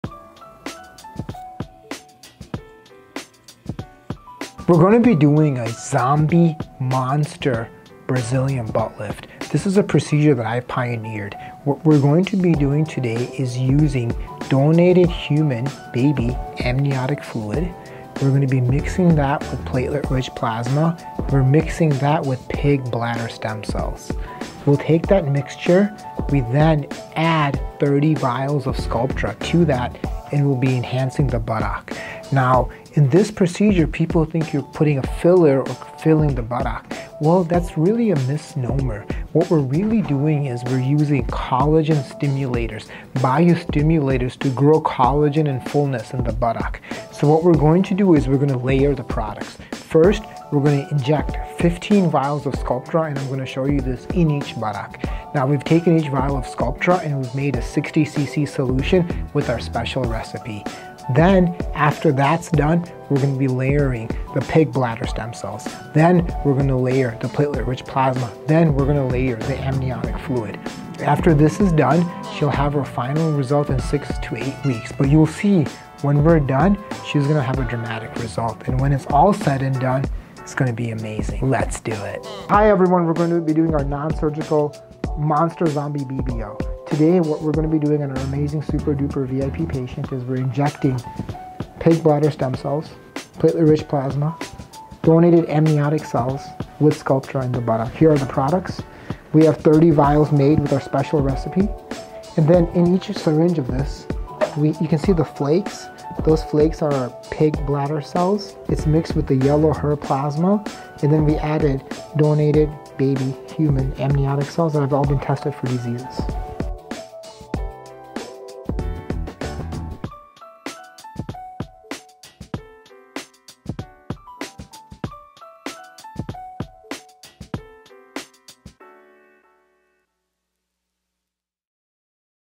Non-Surgical BBL With Fillers 2022
Looking for a non-surgical way to grow and enhance your booty? Dr.Seattle's monster zombie non-surgical BBL may give you a larger size!

Results are variable, not guaranteed and not all patients will be successful with the treatment. Individual results do vary